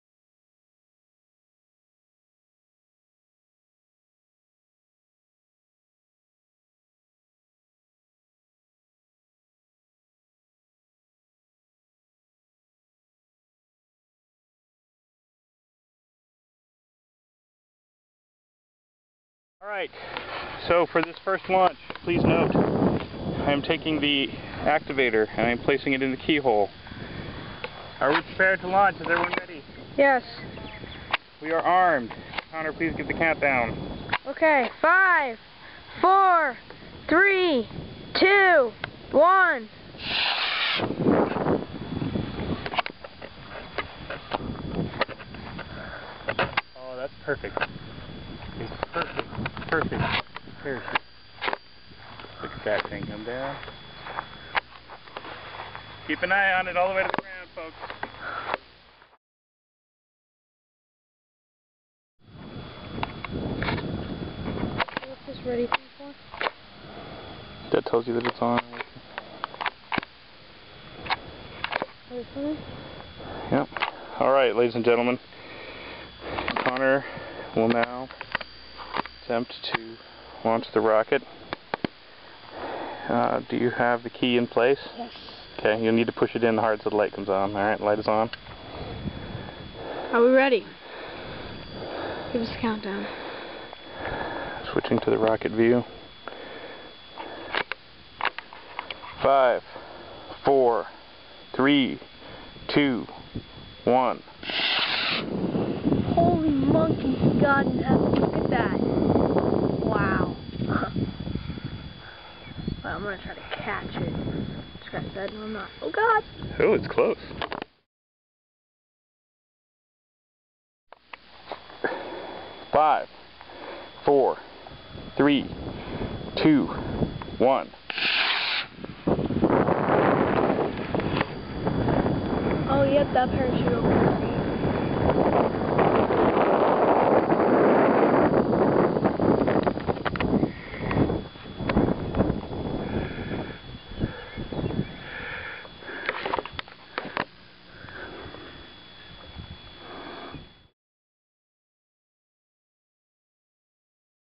Estes Alpha III Launches with On-Board RocketCam!
Lots of Rocket Launches with on-rocket video!